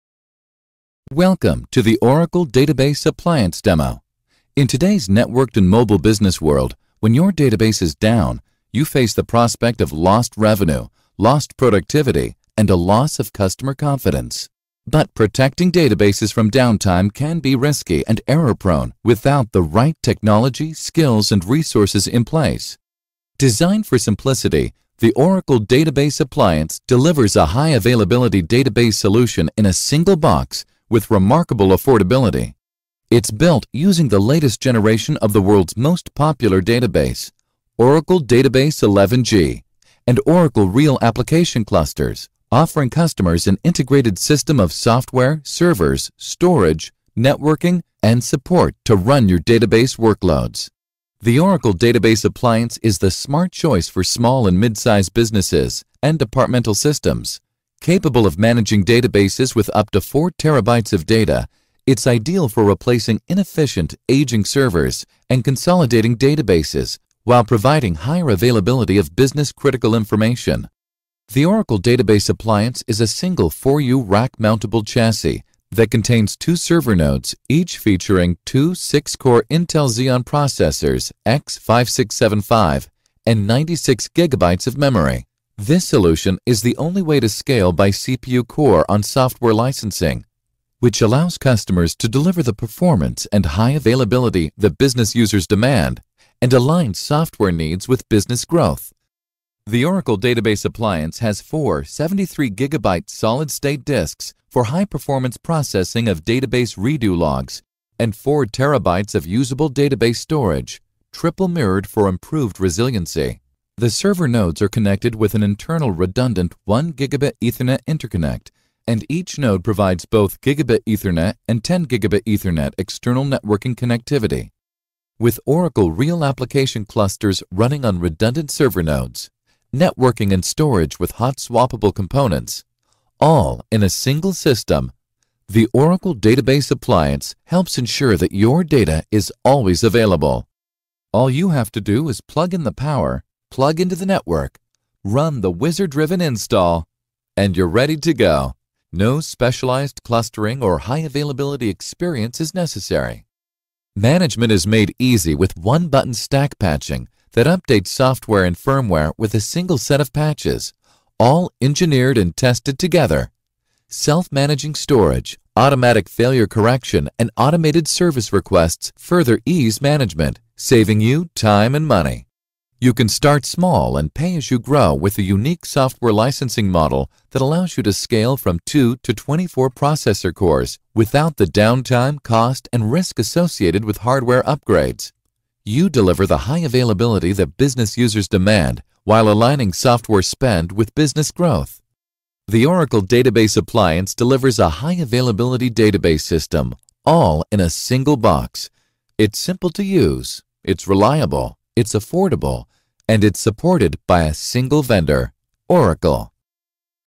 ODA Product Video
Oracle Database Appliance - Simple. Reliable. Affordable.

The Oracle Database Appliance is a new way to take advantage of the world's most popular database—Oracle Database 11g—in a single, easy-to-deploy and manage system. It's a complete package of software, server, storage, and networking that's engineered for simplicity; saving time and money by simplifying deployment, maintenance, and support of database workloads.